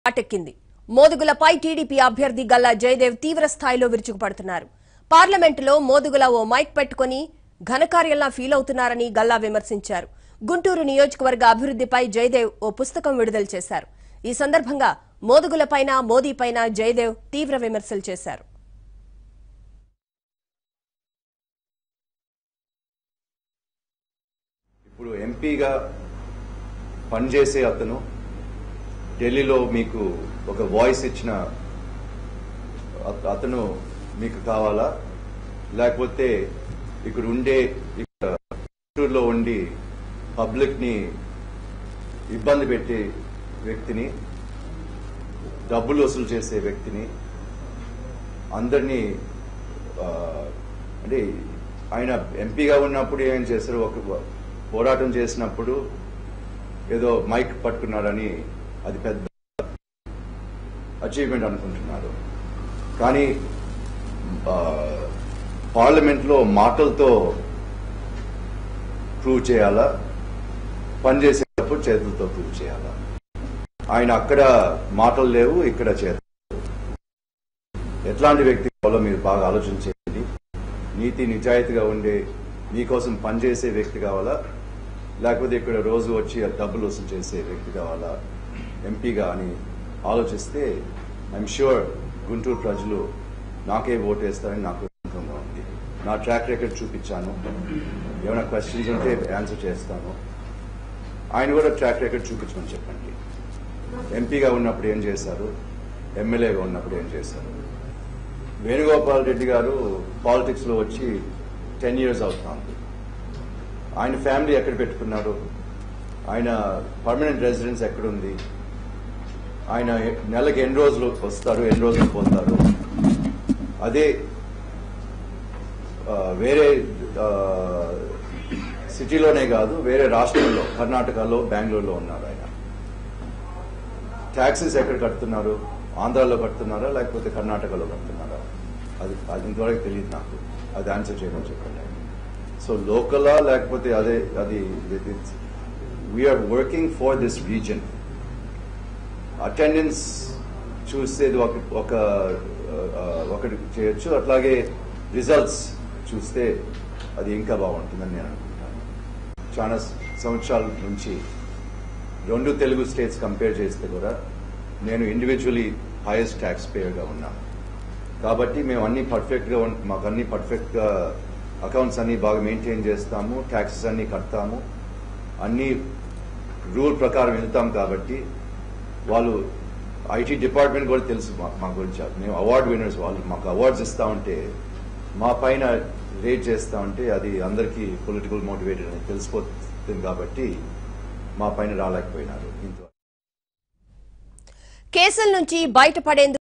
Galla Jayadev Challenges Modugula Venugopala Reddy | ABN Telugu
GallaJayadev challenges ModugulaVenugopalaReddy. He stated that voters have to choose from one who is among the people or a non-local candidate who doesn't know the people's voice. He also said that he is individually the highest tax paid person genuinely among the Telugu states.